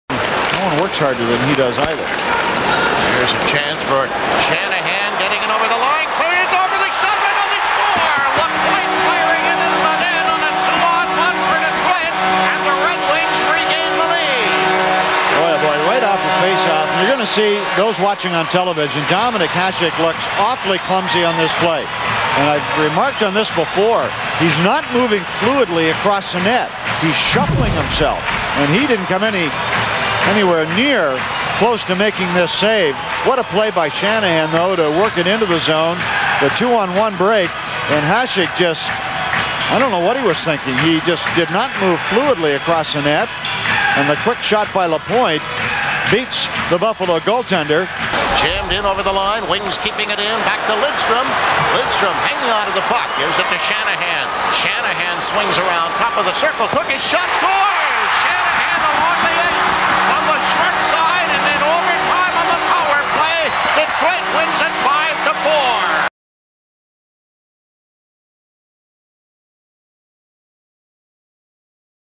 Shanahan sets up Lapointe + OT goal on Hasek (10/21/00 vs Buf)
This was the last time Hasek ever played against Detroit

0:00 - Shanahan makes a nice move around McKee and feeds Lapointe

0:48 - OT Goal. Perfect screen from Holmstrom as usual (No replays available)

Sabres @ Red Wings, October 21st 2000